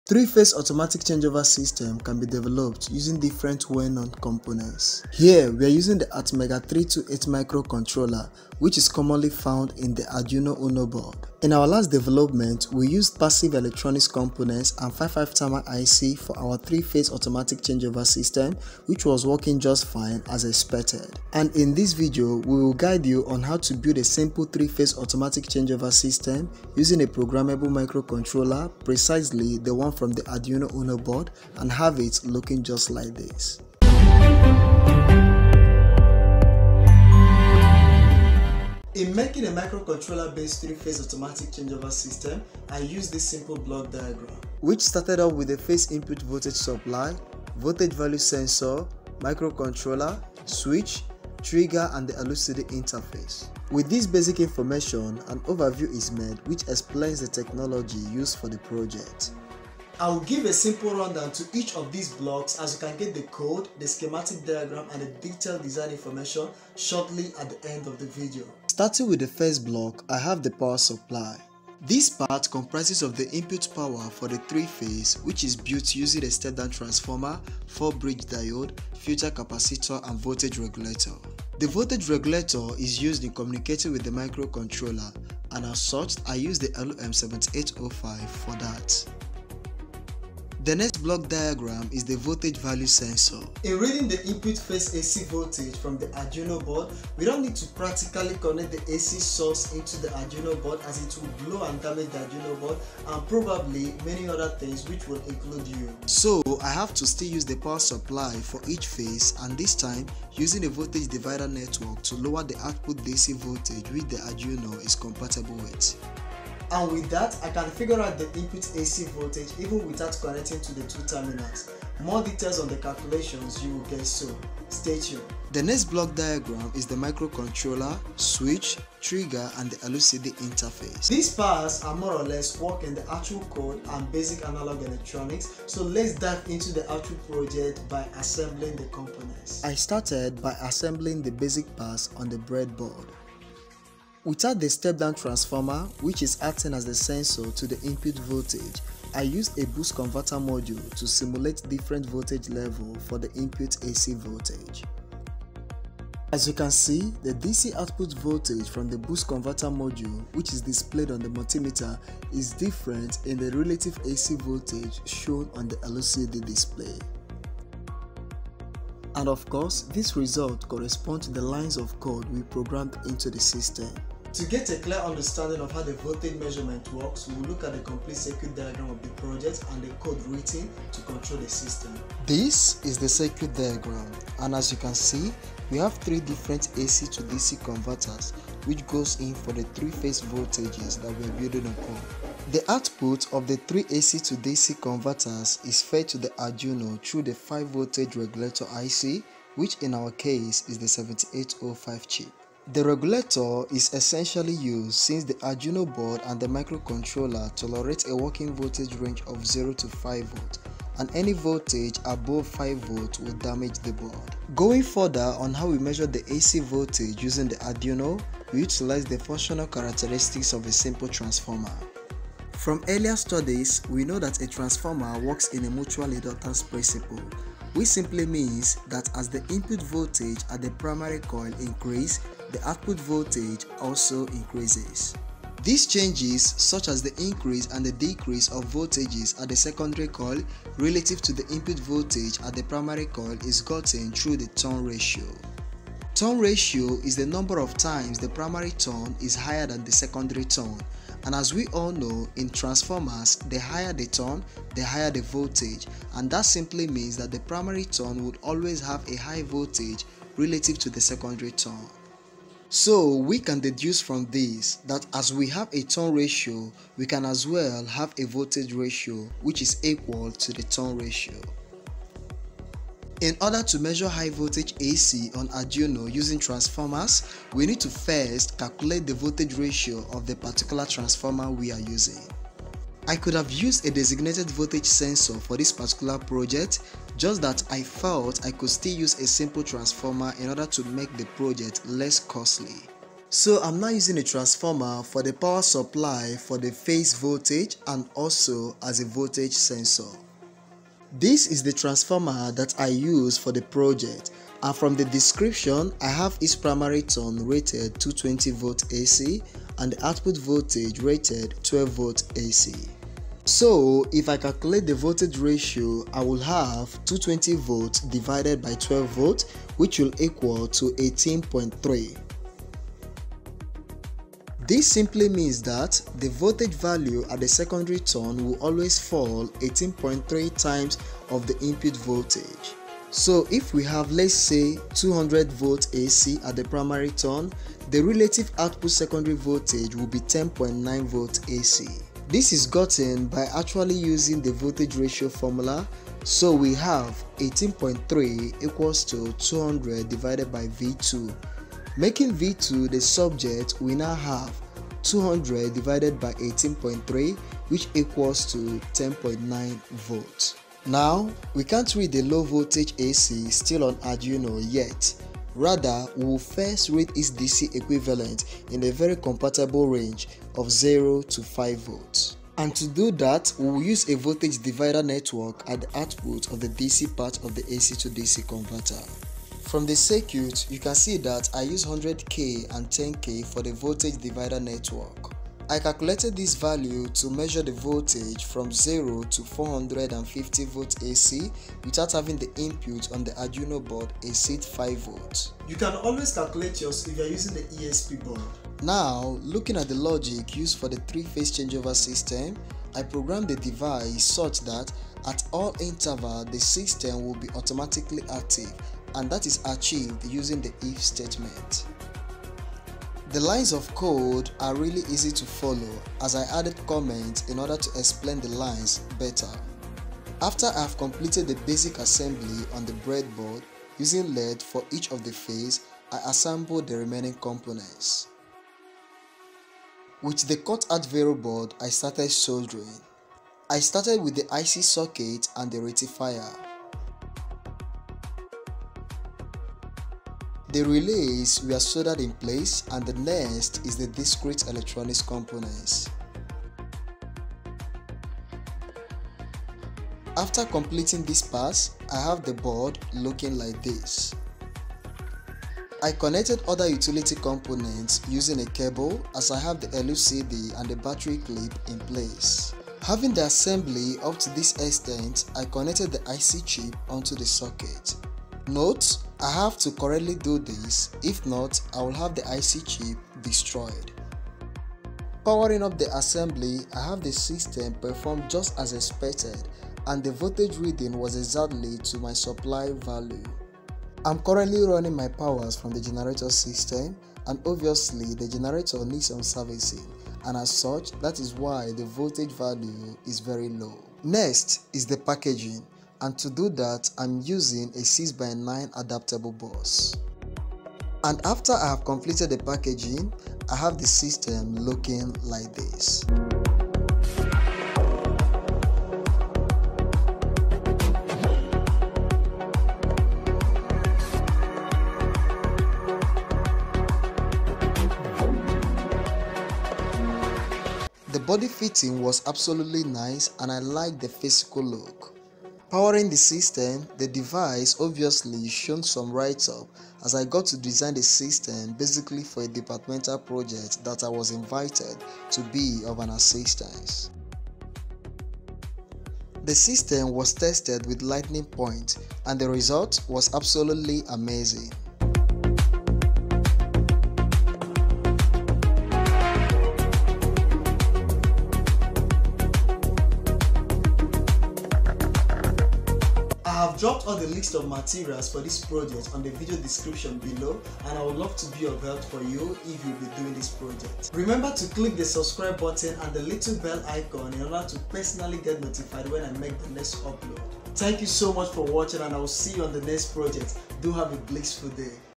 3 PHASE AUTOMATIC CHANGE OVER SYSTEM:How to make an Arduino based 3phase automatic changeover system
Arduino based 3 phase automatic change over system can be handy to build.

In this video, we will share with you the step to step of building your very own 3 phase Automatic change over system, using relay.

The video presents the steps coupled with the design information for this special build.
All the parts can be easily found in any local store.

list of materials:
Arduino uno
relay
10k resistor
100k resistor
1k resistor
crystal oscillator
22pf capacitor
LED 3x
7805 Voltage regulator
9 volts battery
 1N4007 diode
Adaptable box
LCD display 16/2
10k pot

The code and Design information can be found here